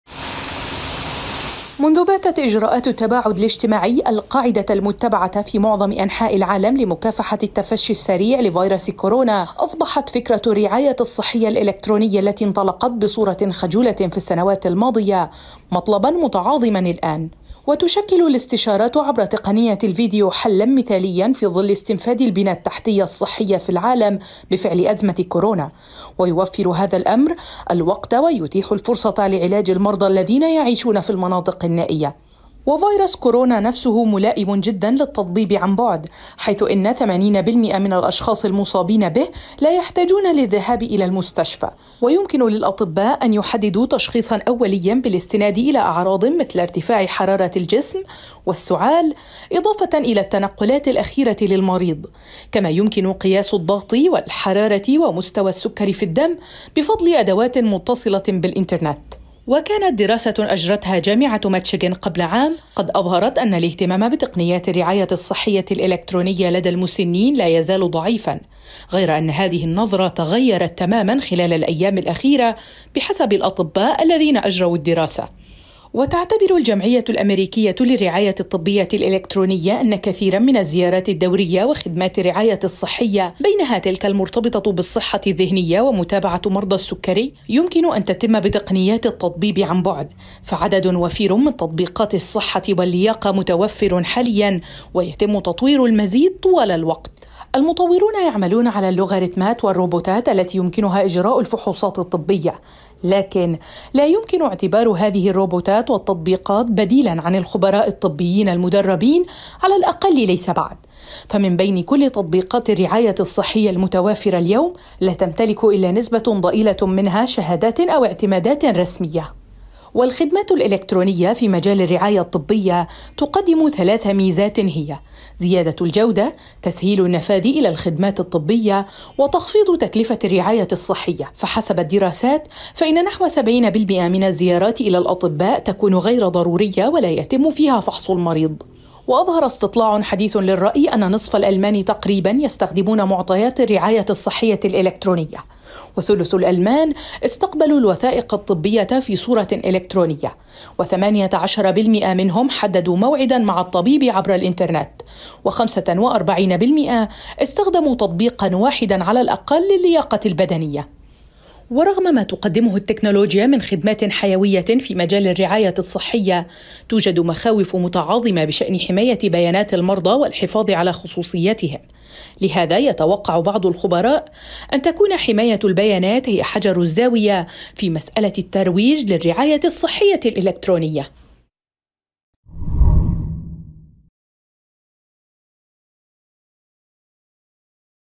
تزايد الاهتمام العالمي بتقنيات الرعاية الصحية الإلكترونية بسبب كورونا
تزايد الاهتمام مؤخرا بسبل الرعاية الإلكترونية بعد التفشي المتسارع لفيروس كورونا عالميا، وتجنب كثير من المرضى التنقل على الطرقات ومواجهة خطر الإصابة بالفيروس في قاعات الانتظار داخل العيادات والمستشفيات.  والاستشارات عبر تقنية الفيديو تمثل حلا مثاليا وتوفر الوقت ونتيح الفرصةَ لعلاج المرضى الذين يعيشون في المناطق النائية... المزيد من التفاصيل عبر التقرير المصور.